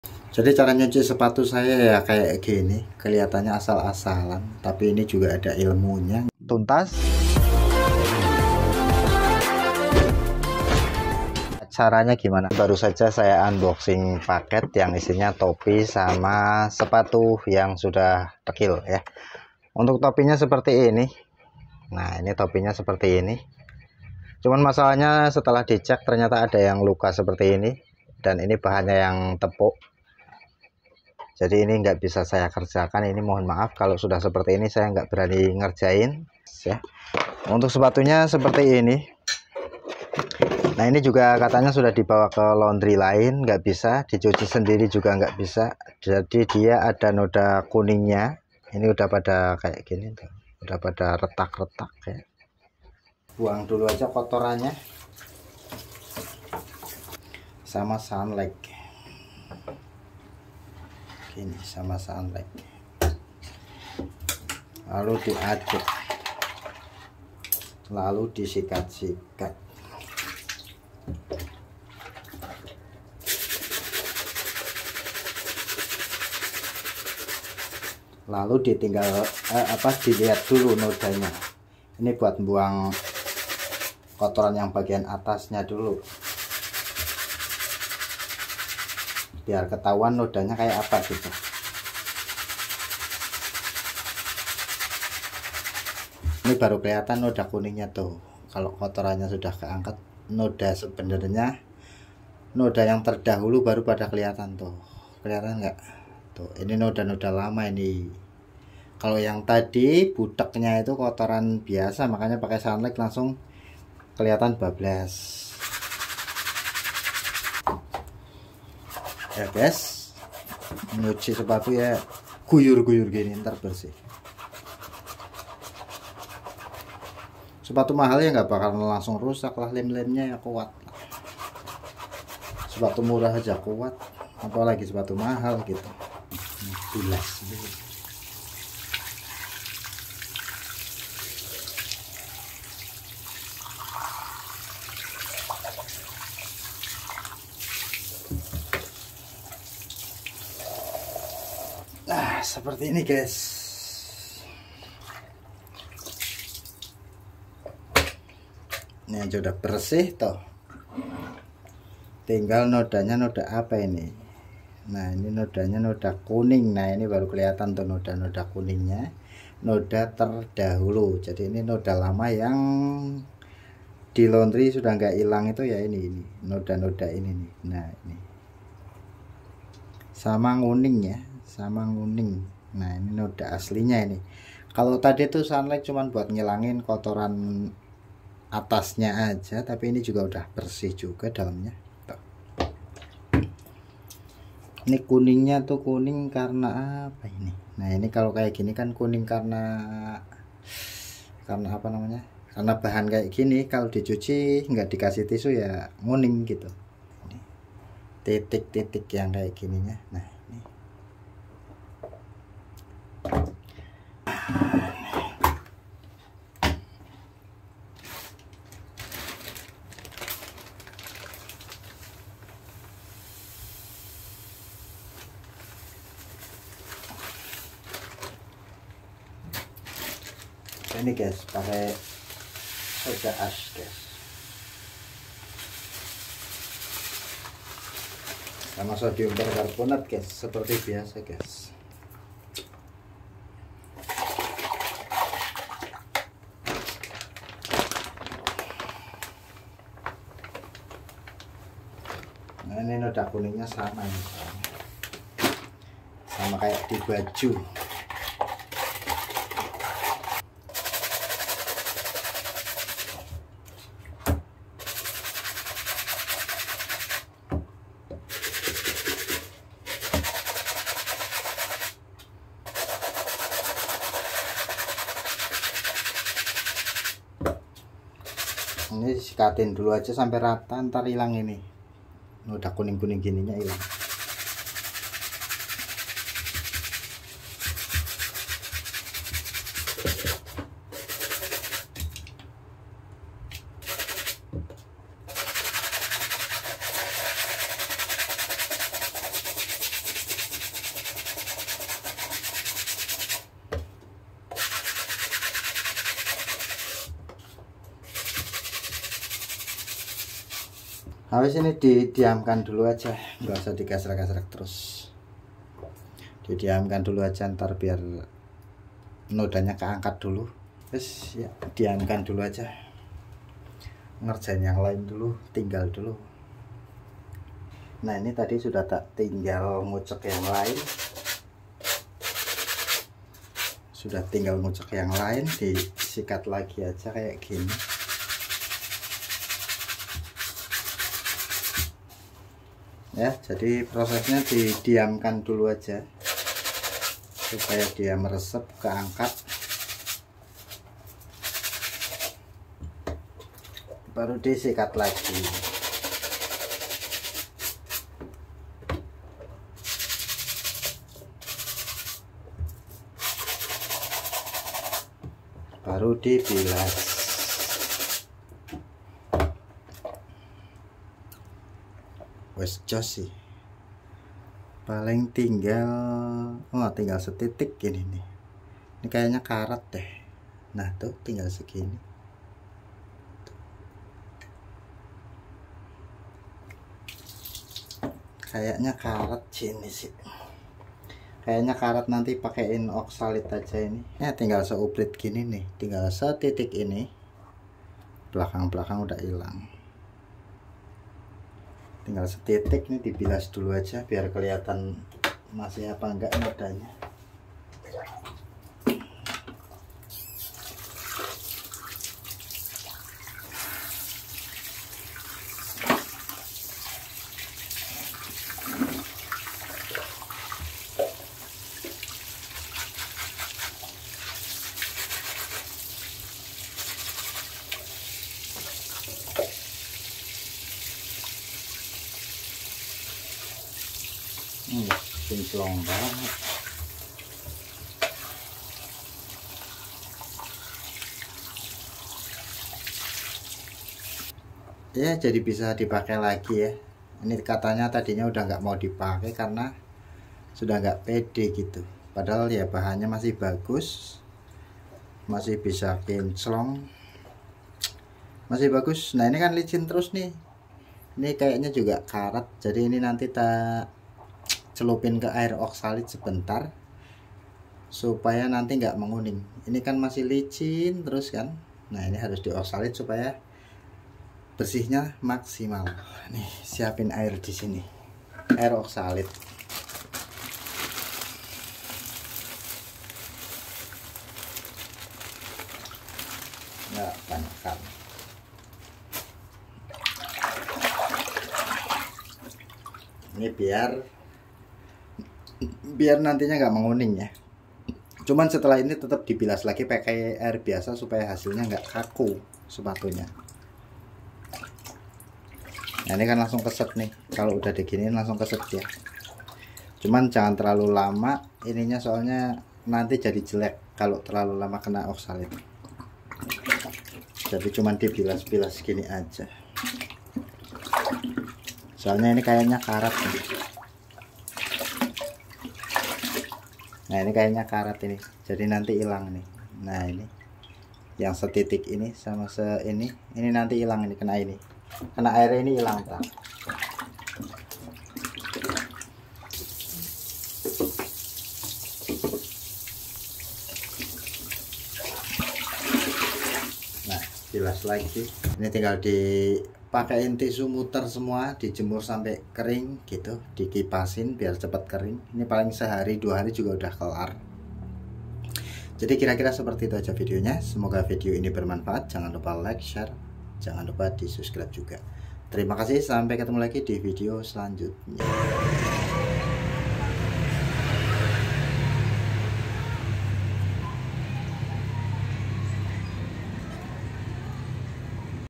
TERBARU MEMUTIHKAN SEPATU MENGUNING DAN KOTOR DI SIMPAN DI GUDANG
cara memutihkan sepatu yang menguning,kotor dekil karna di simpan lama di gudang,di simpan dalam keadaan kotor karna awalnya di cuci noda kuningnya gak hilang,katanya sudah males pakai dan alhamdulilah dibawa kesini masih bisa bersih
silahkan simak videonya ada 3 tahap supaya hasilnya bersih gak menguning setelah di cuci